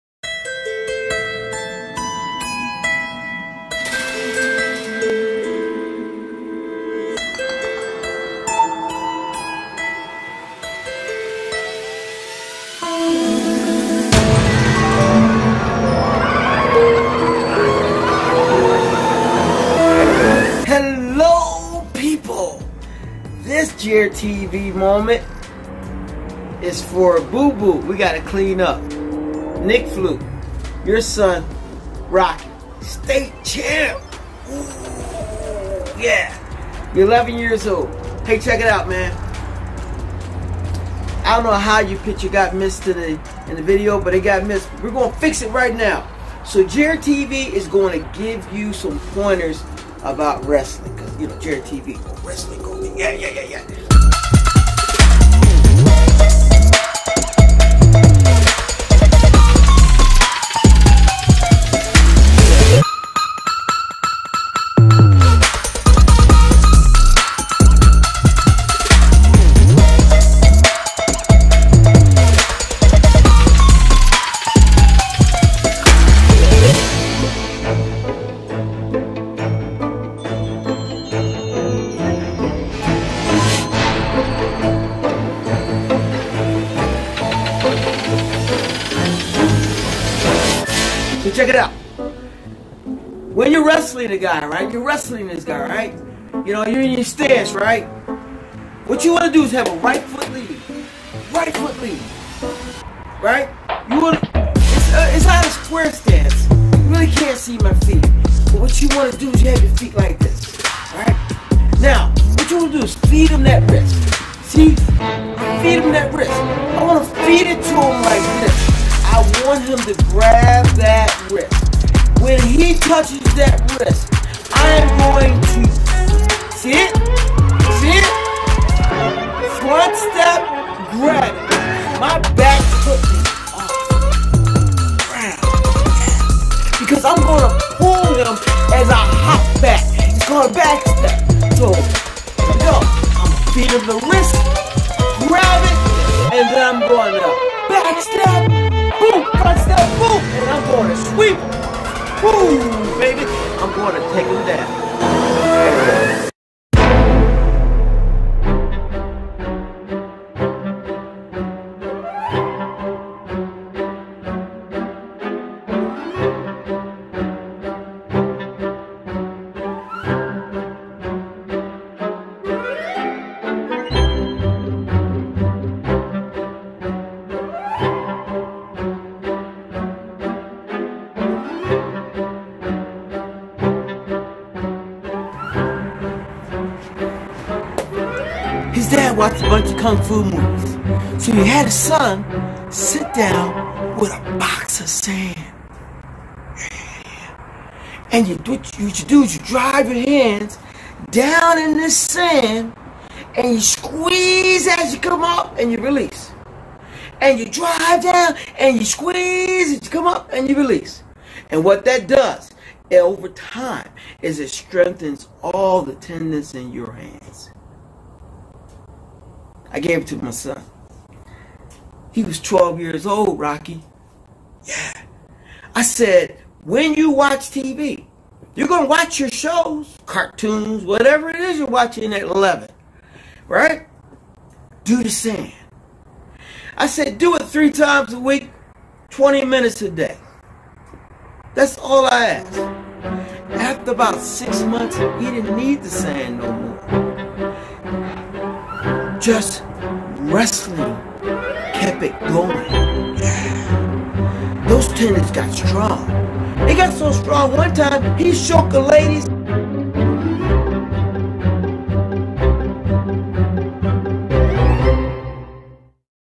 Rocky's Wrestling Tip
How to get your hands strong, wrestling techniques